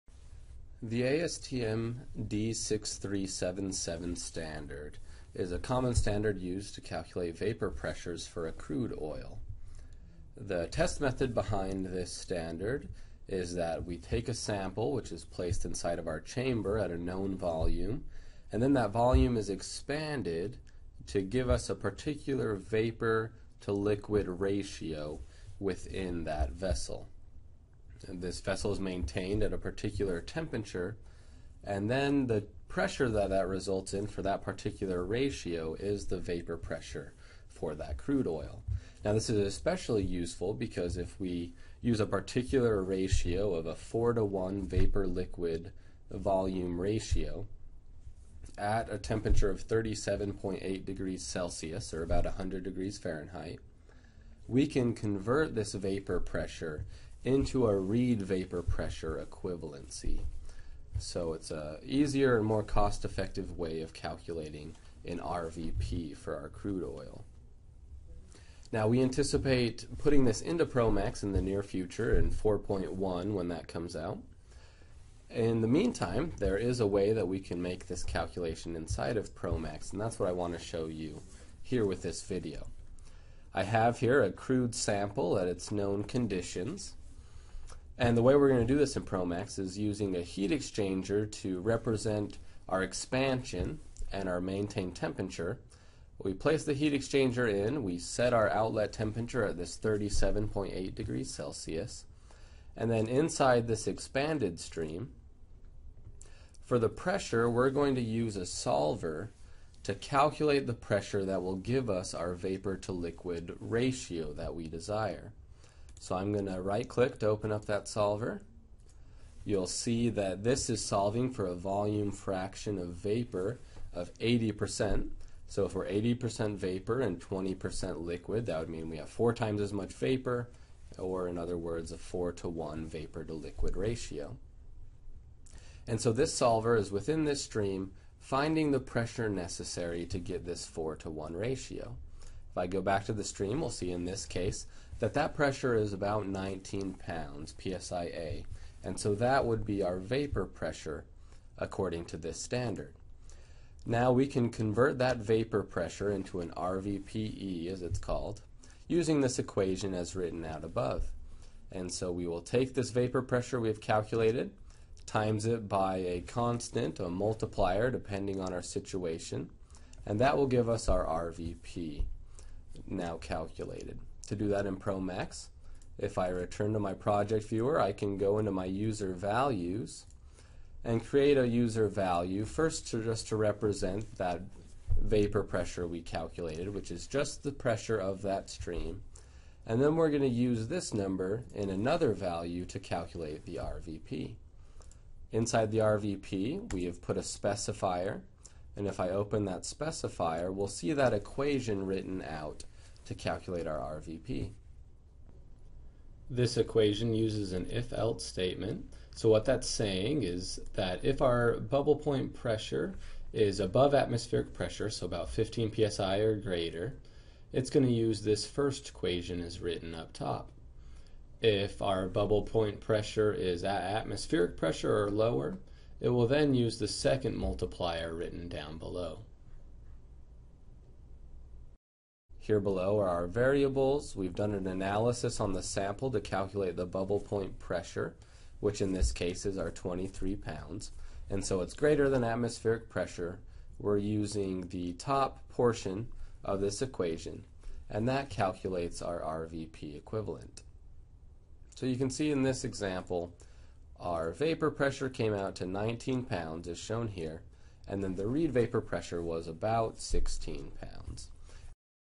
D6377 Vapor Pressure to RVP
This video describes how to perform the ASTM D6377 standard to calculate vapor pressure of a crude oil. The video also shows how at a 4:1 vapor-liquid volume ratio you can convert this vapor pressure into a Reid Vapor Pressure equivalent.